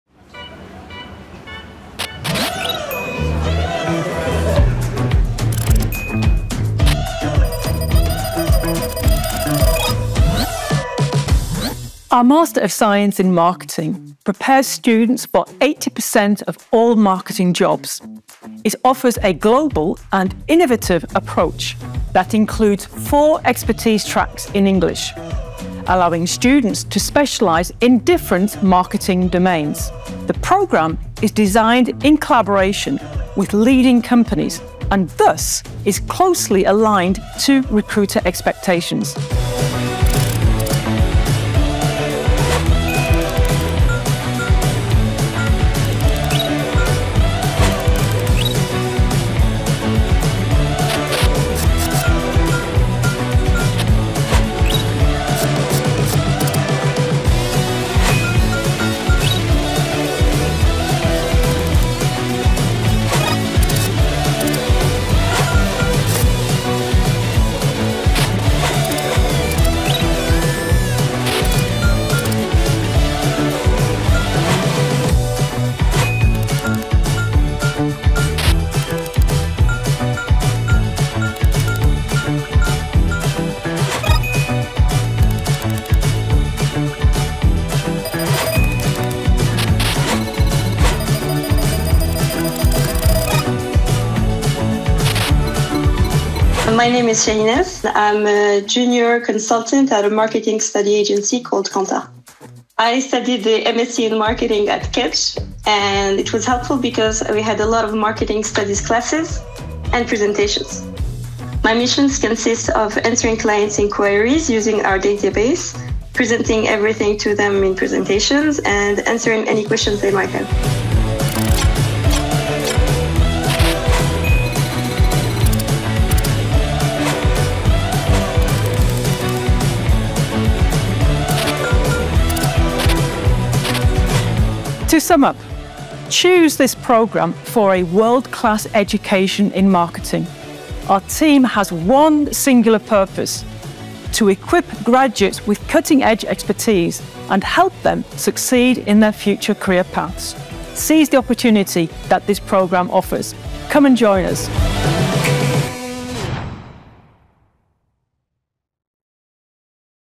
MSc, Master of Science Marketing - Programme presentation
Available in Bordeaux, Paris and Marseille, the MSc 'Marketing' offers an innovative approach to marketing, combining theoretical knowledge with advanced practical skills. Over a one- to two-year course, the programme covers numerous themes and tools related to the latest developments in the industry. Students have a choice between 4 key specialisations - all approaching marketing as a source of value creation. Graduates progress to a wide range of positions in this field worldwide.

Disponible à Bordeaux, Paris et Marseille, le MSc 'Marketing' propose une approche innovante du secteur, associant connaissances théoriques et pratiques avancées. Sur un cursus d’un à deux ans, le programme couvre un grand nombre de thèmes et d’outils liés aux dernières évolutions de l'industrie. Les étudiants ont le choix parmi 4 spécialisations clé - qui abordent le marketing comme source de création de valeur. Une fois diplômés, ils seront en mesure d’évoluer dans un large panel de postes dans le marketing à l'échelle mondiale.